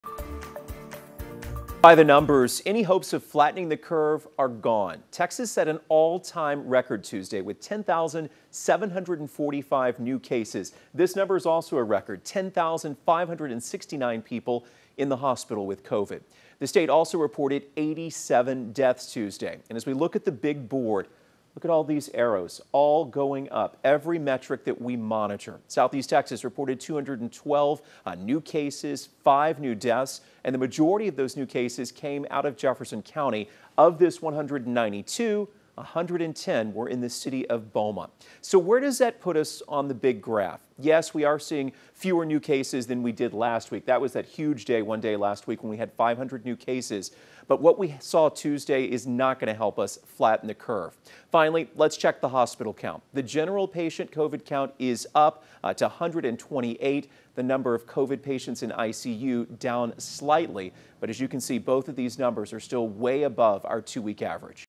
COVID-19 by the numbers: July 14
Texas' numbers were up Tuesday, including hospitalizations and new cases